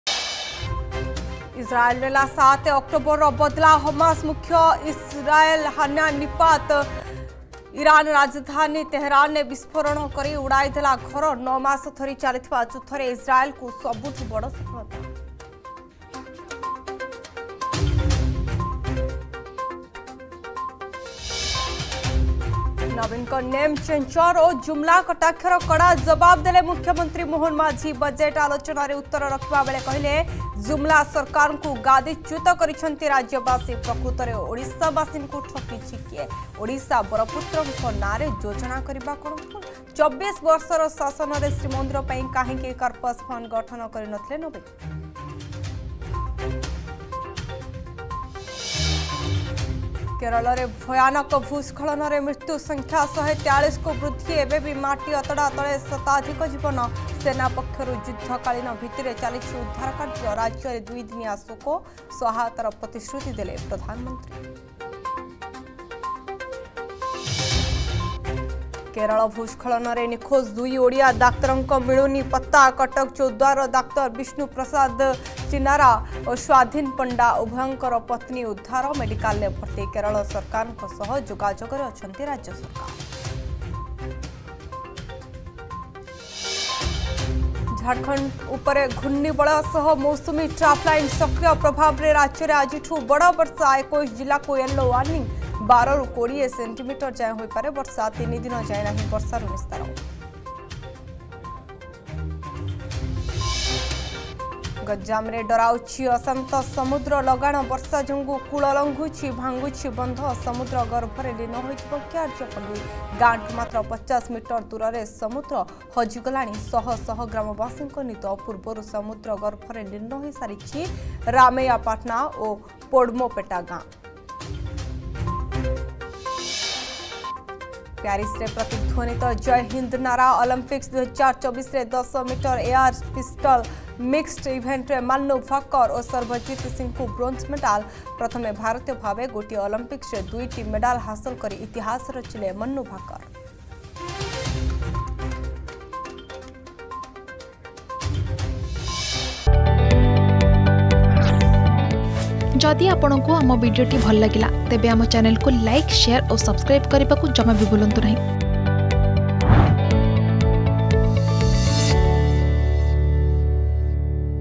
Headlines 10am || 31 Jul 2024 || Argus News
+ଇସ୍ରାଏଲ୍ ନେଲା 7 ଅକ୍ଟୋବରର ବଦଲା
+ ନବୀନଙ୍କ ନେମ୍ ଚେଞ୍ଜର ଓ ଜୁମଲା କଟାକ୍ଷର କଡ଼ା ଜବାବ ଦେଲେ ମୁଖ୍ୟମନ୍ତ୍ରୀ ମୋହନ ମାଝୀ
+କେରଳରେ ଭୟାନକ ଭୂସ୍ଖଳନରେ ମୃତ୍ୟୁ ସଂଖ୍ୟା 143କୁ ବୃଦ୍ଧି
+ କେରଳ ଭୂସ୍ଖଳନରେ ନିଖୋଜ ଦୁଇ ଓଡ଼ିଆ ଡାକ୍ତରଙ୍କ ମିଳୁନି ପତ୍ତା
+ଝାଡ଼ଖଣ୍ଡ ଉପରେ ଘୂର୍ଣ୍ଣିବଳୟ ସହ ମୌସୁମୀ ଟ୍ରାପ୍ ଲାଇନ୍ ସକ୍ରିୟ
+ ଗଞ୍ଜାମରେ ଡରାଉଛି ଅଶାନ୍ତ ସମୁଦ୍ର